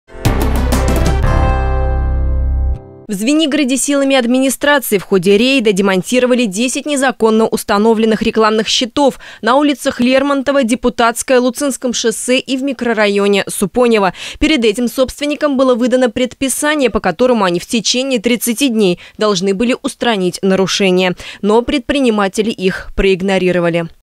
В Звенигороде демонтировали незаконные рекламные конструкции.
В Звенигороде силами администрации демонтировали 10 незаконно установленных рекламных щитов на улицах Лермонтова, Депутатская, Луцинском шоссе и в микрорайоне Супонево.